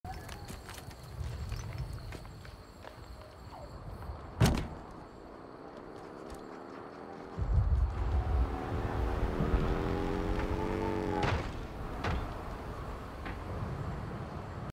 The Wrong Kind of Chopper
This brute thought his chopper could fly. Nobody had the heart to tell him that he was in the wrong type of chopper.